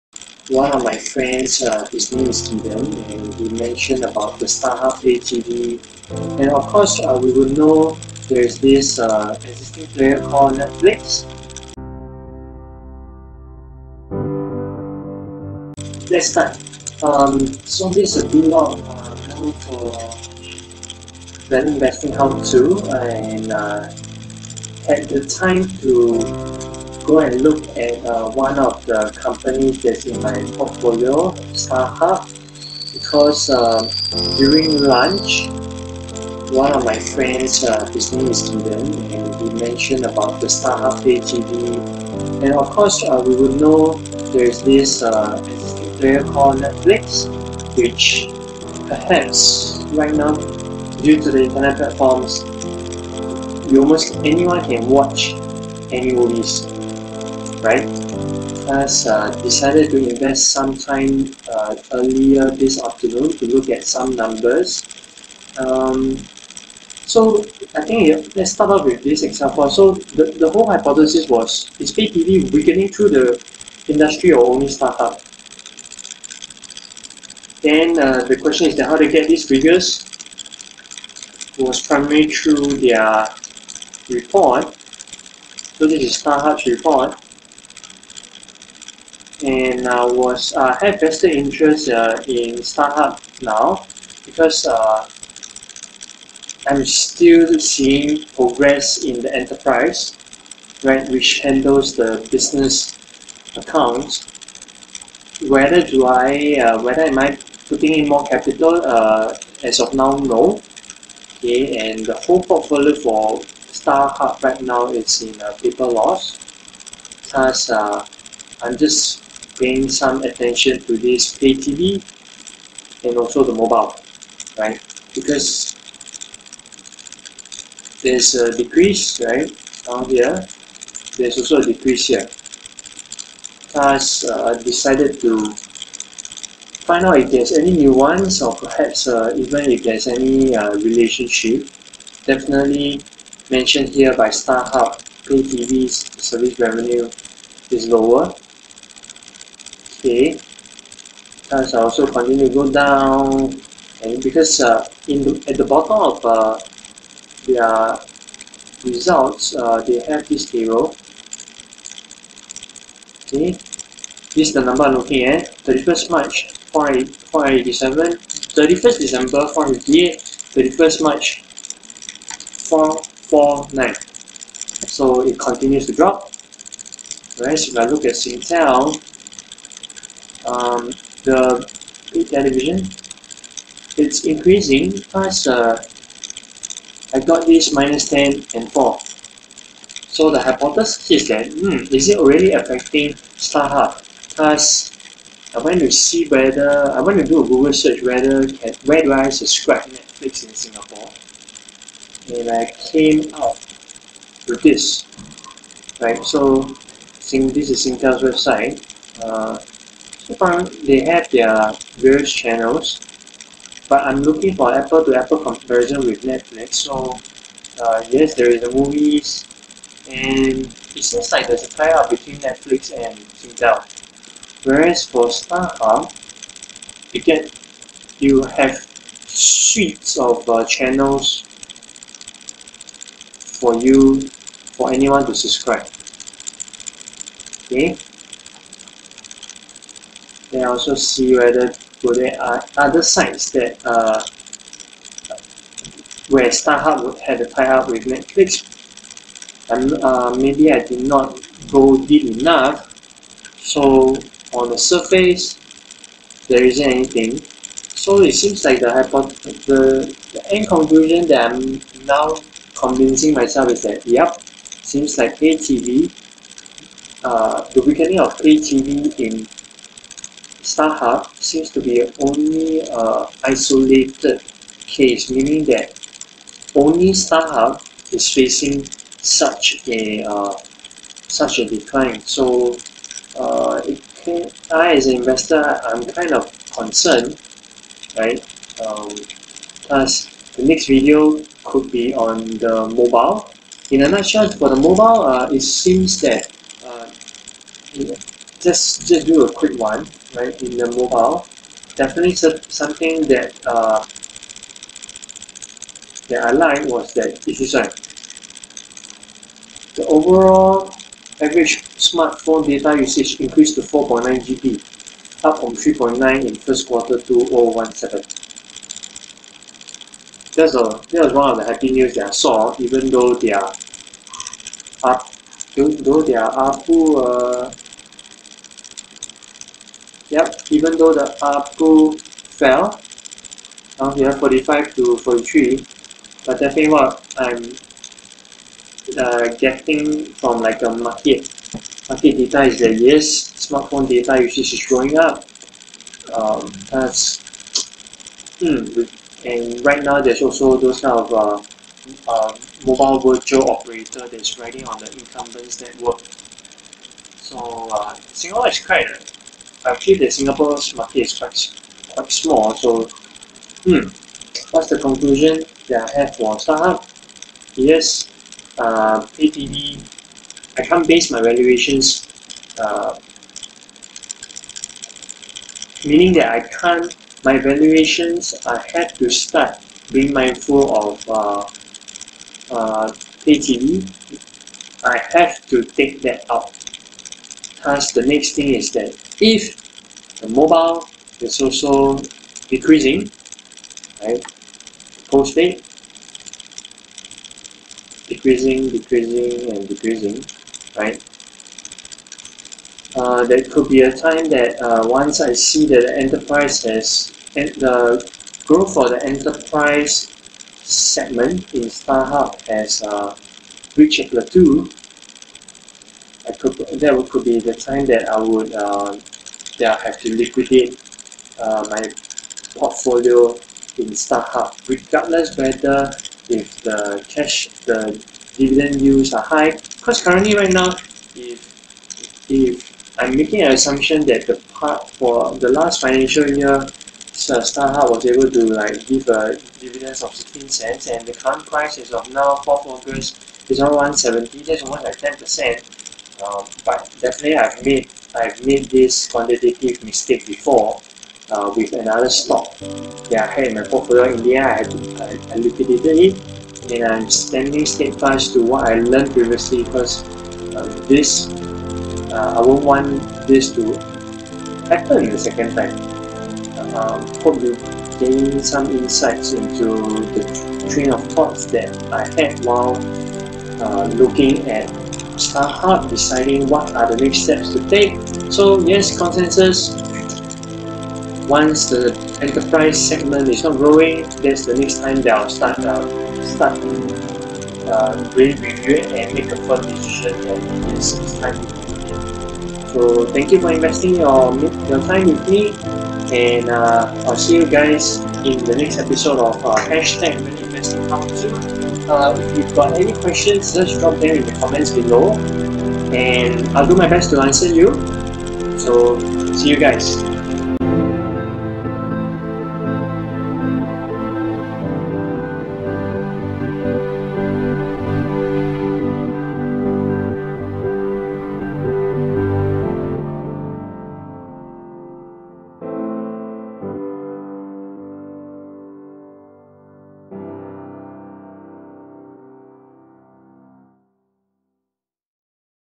[Value Investing How-to] 004.2 Reviewing My Starhub Pay TV Business Segment
How did this video on Starhub Pay TV come about?

I got nervous when my friend, Gideon, raised his point on paid television and Netflix. For the years I've a vested interest in Starhub, I always knew that that division numbers are decreasing till I decided to invest time to review this paid television.

Did you invest in Starhub too? Drop your comments below if you wish to share your point of view. I'll be reading them.

I hope you keep up with the videos I post on the channel. Say "Hey" in the comments below so that I know that my video has reached you.